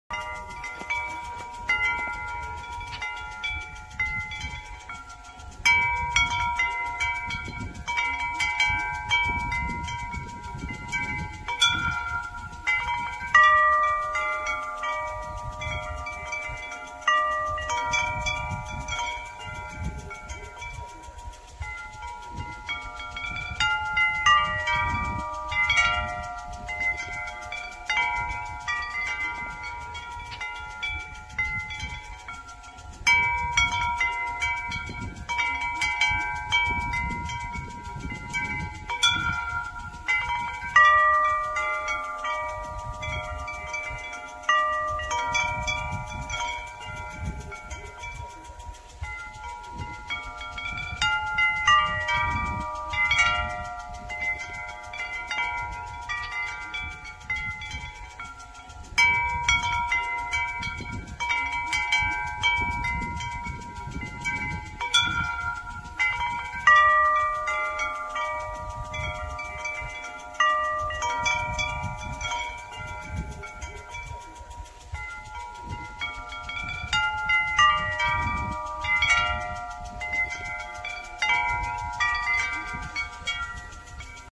Apricot Cottage Meditation Moment - Apricot Blossoms🌸
This morning the wind is a gentle player of my wind chime. The tree is overflowing with blossoms and the air is full of their fragrance.  I wanted to share a few minutes with you in the sunshine of my front porch.